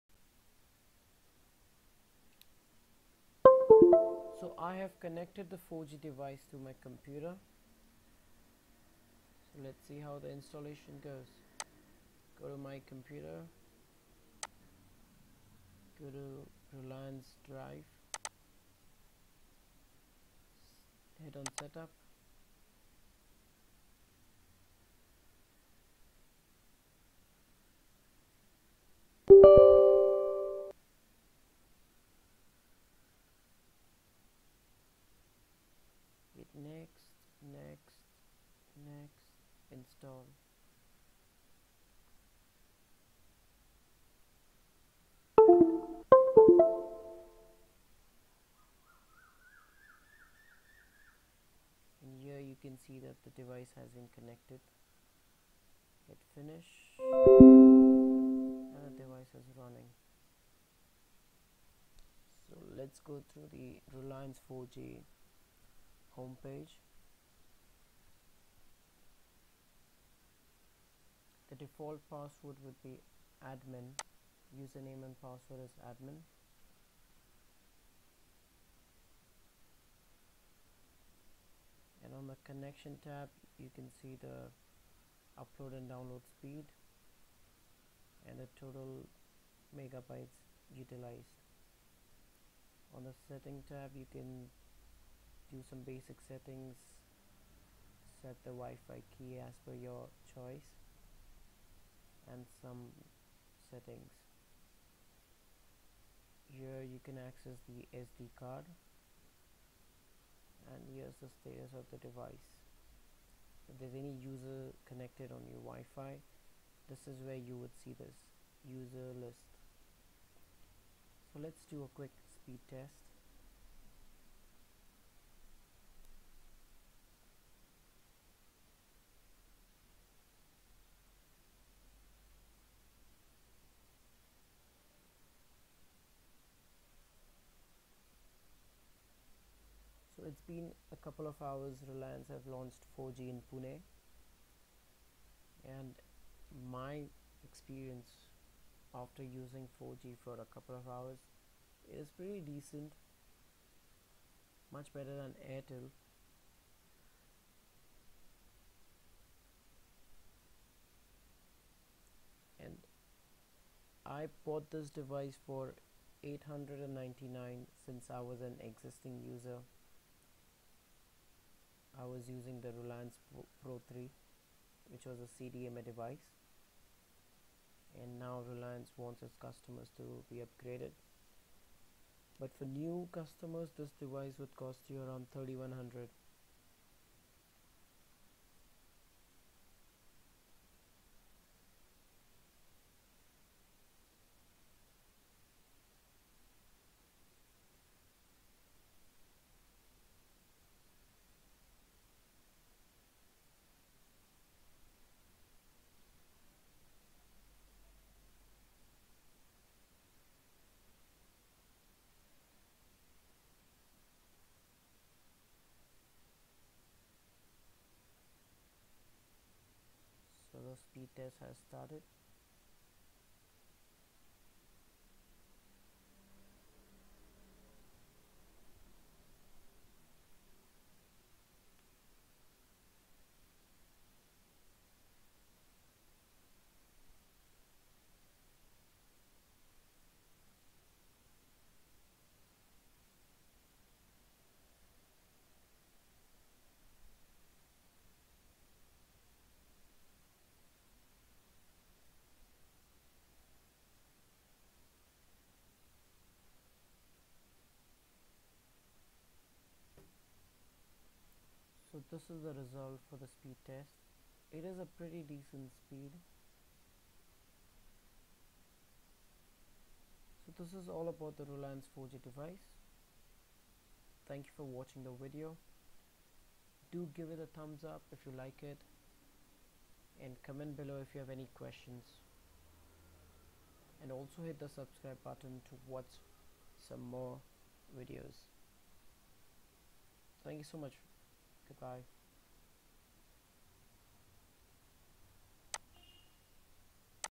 Reliance 4G Wi-Pod LTE installation, reviews and speed test.
Reliance Communications has upgraded it's CDMA customer's to 4G network. Here's a short review about the 4G device.

It is a decent device and low quality chinese product. The device which is manufactured by ZTE weighs only 80g and yet feels more premium than the Jio Mi-Fi. The front face has the power button along with the Reliance Wi-Pod branding and 5 Led indicators for LTE signal strength, uplink and downlink data transmission, wifi network, WPS and battery charge level. The right panel has the familiar micro USB charging and connectivity port and the left panel has a WPS key to activate WPS(Wi-Fi Protected Setup)